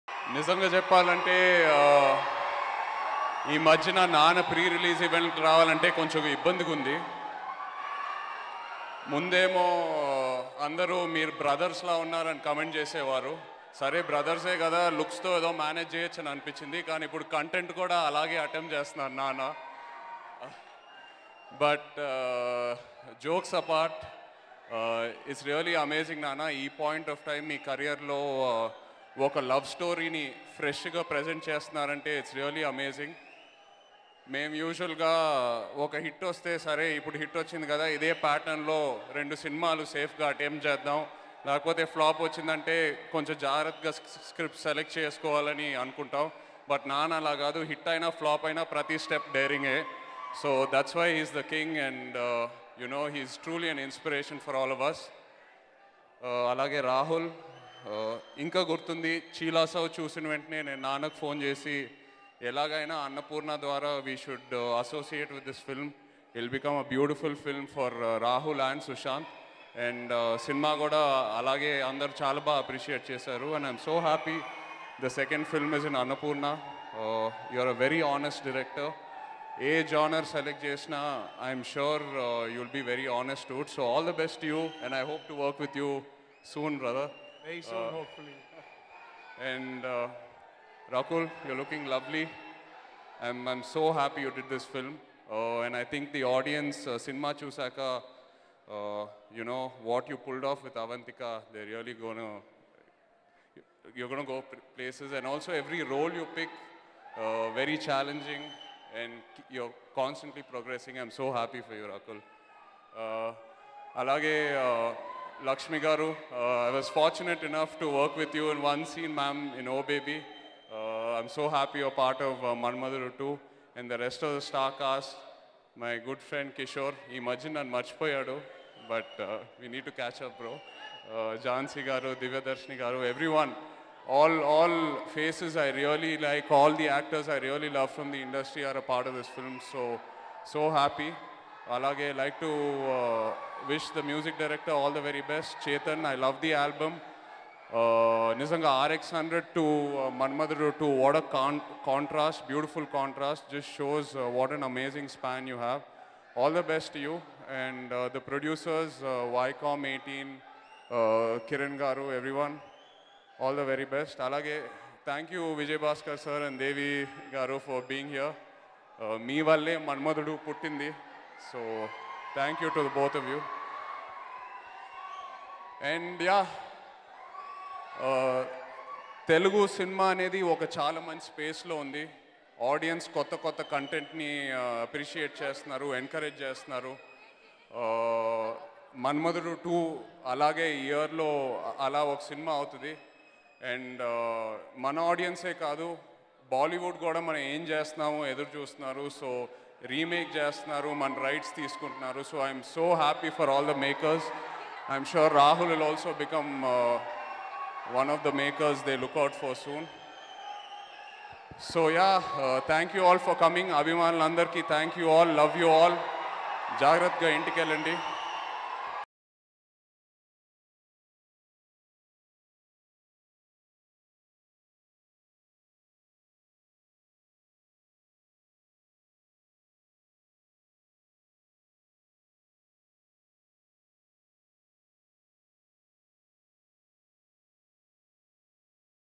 Naga Chaitanya Hilarious Speech @Manmadhudu 2 Pre-Release Event | Niharika Movies
Watch Naga Chaitanya Hilarious Speech @Manmadhudu 2 Pre-Release Event.